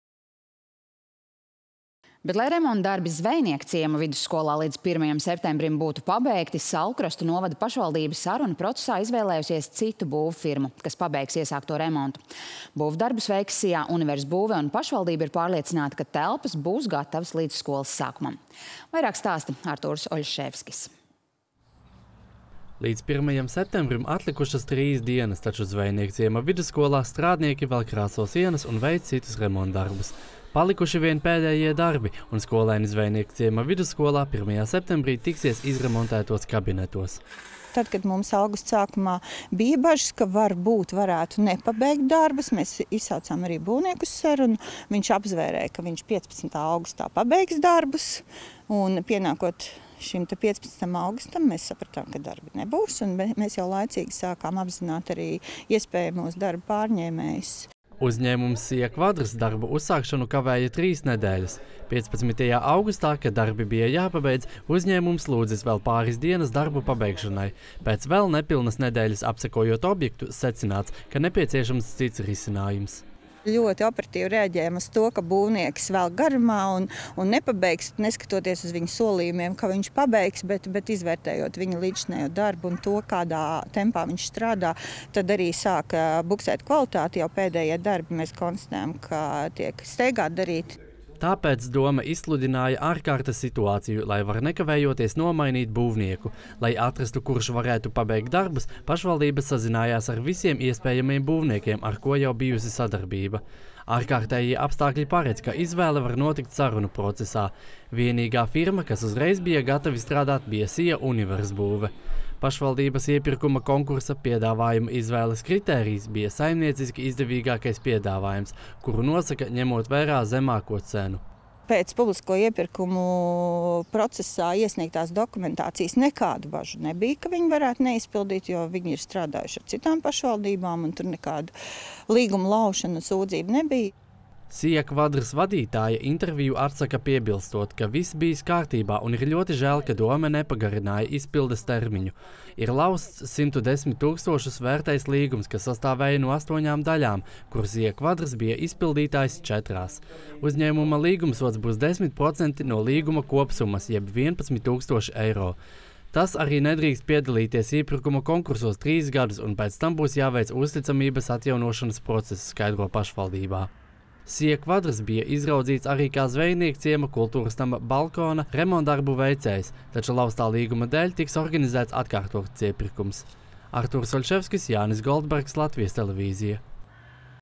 Nomaina remontdarbu izpildītāju Zvejniekciema vidusskolā
Lai remontdarbi Zvejniekciema vidusskolā līdz pirmajam septembrim būtu pabeigti, Saulkrastu novada pašvaldība sarunu procesā izvēlējusies citu būvfirmu, kas pabeigs darbus. Tagad darbus veiks “SIA Universbūve” un pašvaldība ir pārliecināta, ka telpas būs gatavas līdz skolas sākumam. Vairāk stāsta Artūrs Oļševskis.